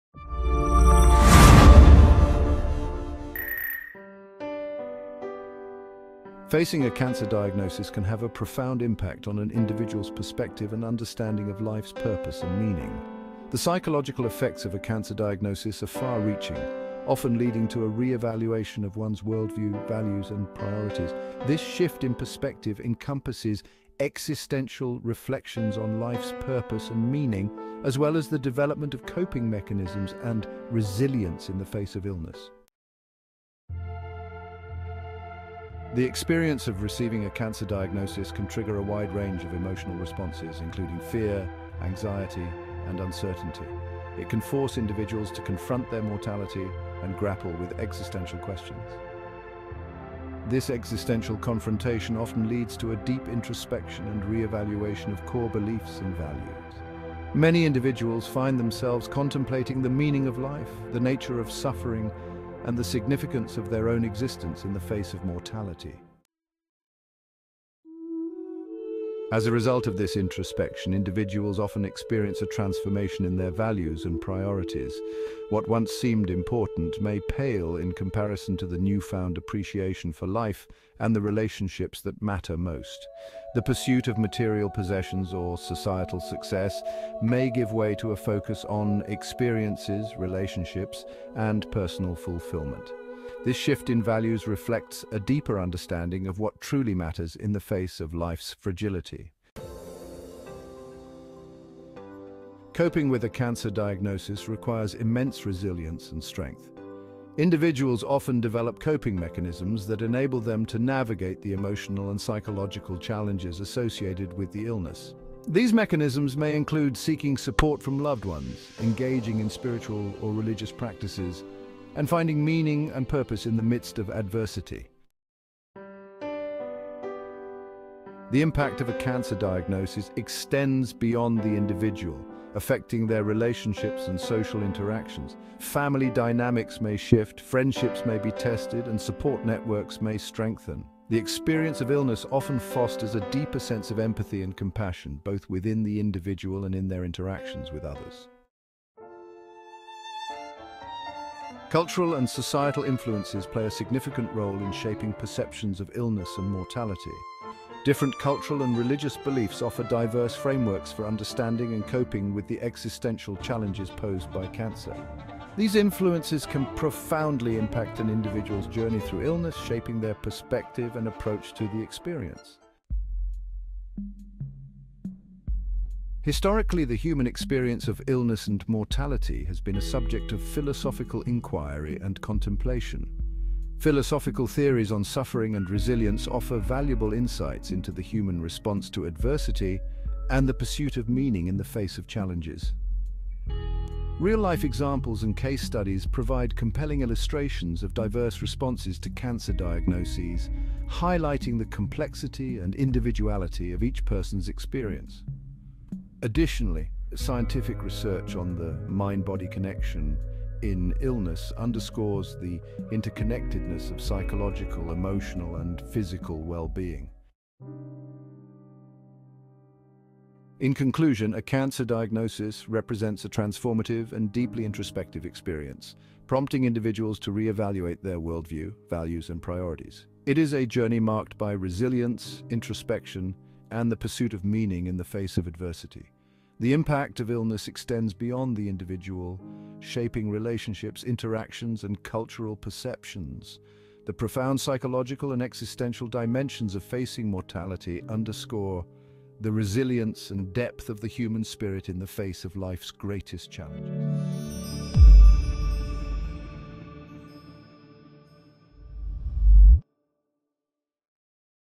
The Impact of Illness on Perspective
A cancer diagnosis can significantly alter an individual's perspective on life's purpose and meaning. It can trigger emotional responses such as fear, anxiety, and uncertainty, leading to a deep introspection and re-evaluation of core beliefs and values. This introspection often results in a transformation in values and priorities, shifting from material possessions to experiences, relationships, and personal fulfillment. Coping with a cancer diagnosis requires resilience and strength, as individuals often develop coping mechanisms such as seeking support from loved ones and engaging in spiritual practices. The experience also impacts relationships and social interactions, affecting family dynamics, friendships, and support networks. Cultural and societal influences also shape perceptions of illness and mortality. Philosophical theories on suffering and resilience offer insights into the human response to adversity and the pursuit of meaning in the face of challenges. In conclusion, a cancer diagnosis is a transformative and deeply introspective experience, requiring resilience, introspection, and the pursuit of meaning in the face of adversity.